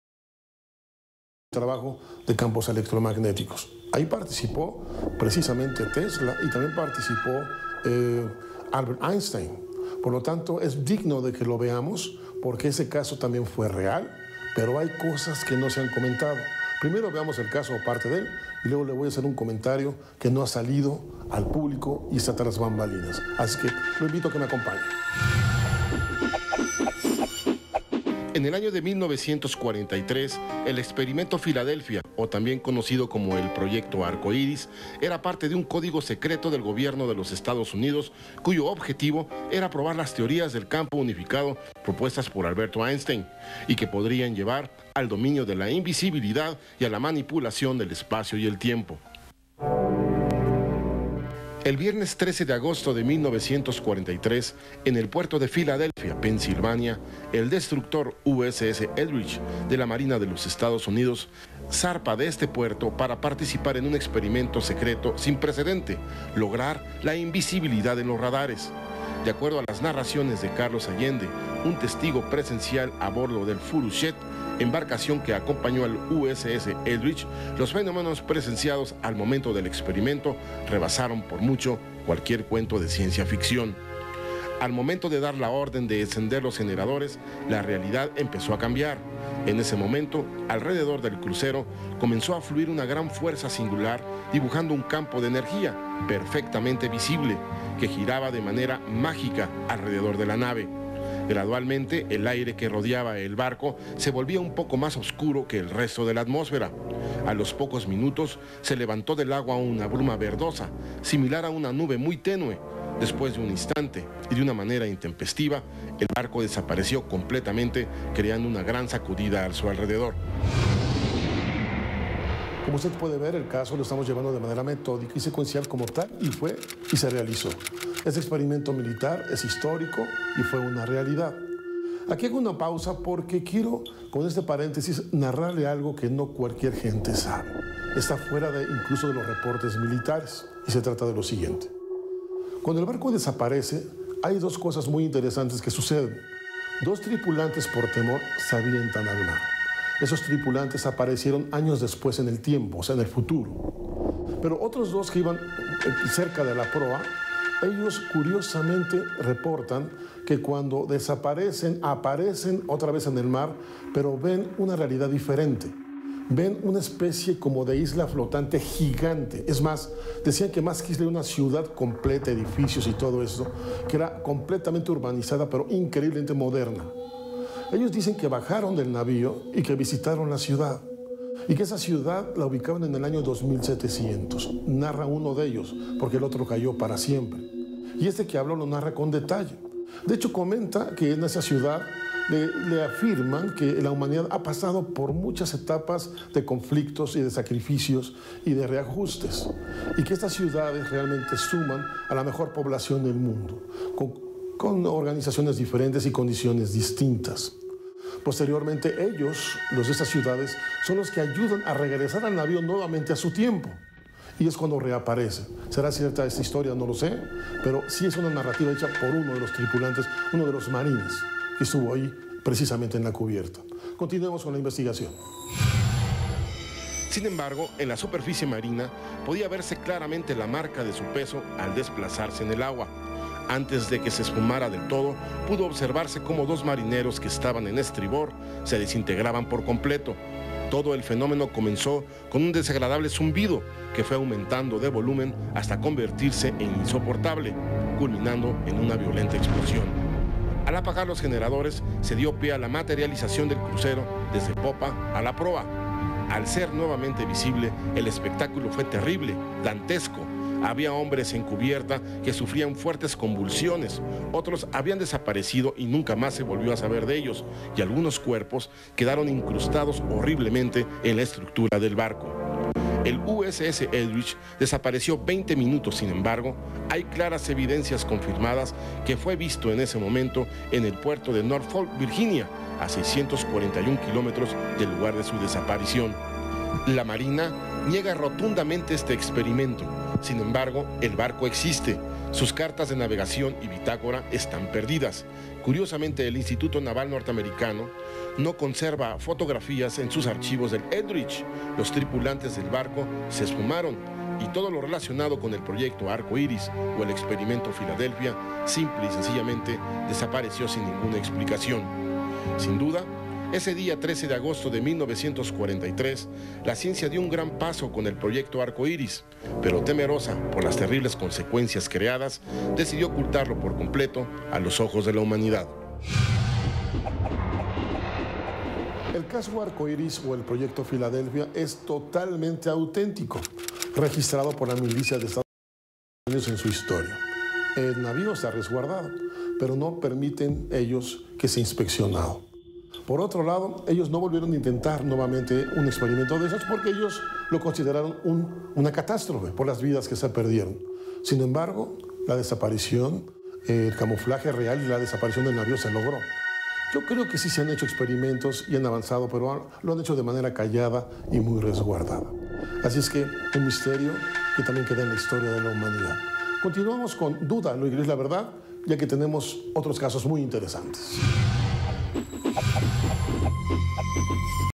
HBO Sobrevivientes del proyecto filadelphia explican sus visiones del año 2700 Ciudades Flotantes
Discovery Cannel Sobrevivientes del proyecto filadelphia explican sus visiones del año 2700 las ciudades flotantes en estados unidos de america